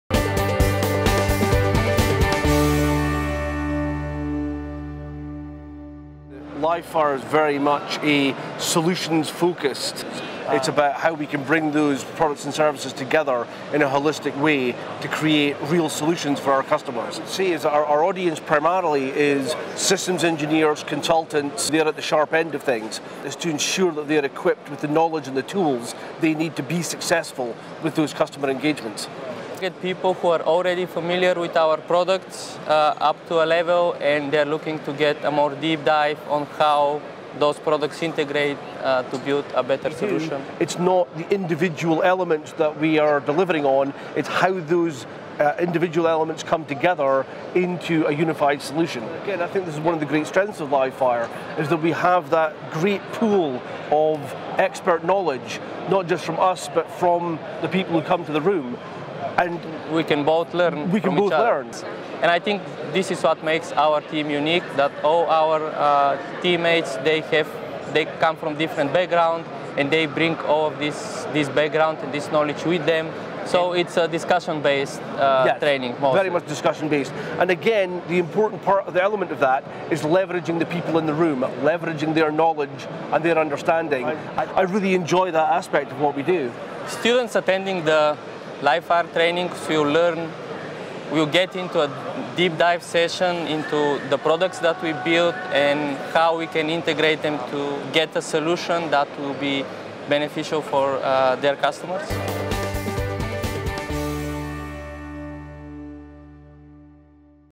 VMware Livefire Workshops – what are they?
Learn about the key features of Livefire training, who should attend and the benefits gained from attending these expert-to-expert workshops. For datasheets, course overviews and upcoming VMware Livefire workshops check out